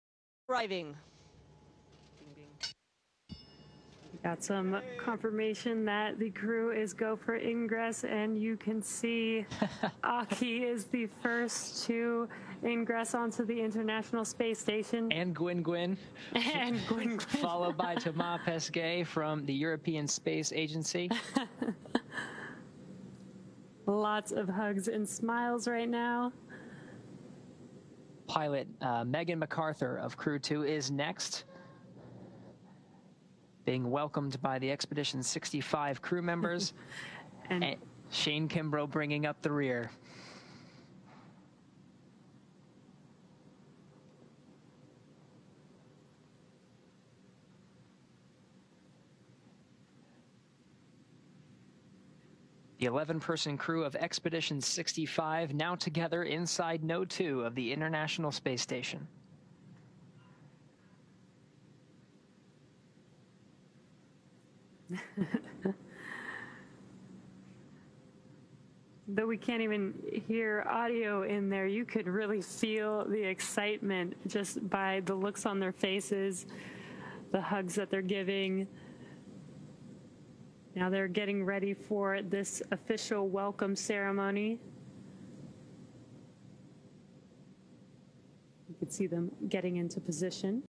Welcome To the ISS : Crew 2 astronauts arrived to the ISS .Crew 2 astronauts enters ISS. Nasa,Spacex
The astronomers of crew 2 dragon endeavour finally reached international space station, and entering to the iss where on board astronauts are welcoming them. every is hugging each other's, what a moment, enjoy the magical moment and
help me to reach 1000 subscribers at the end of this month.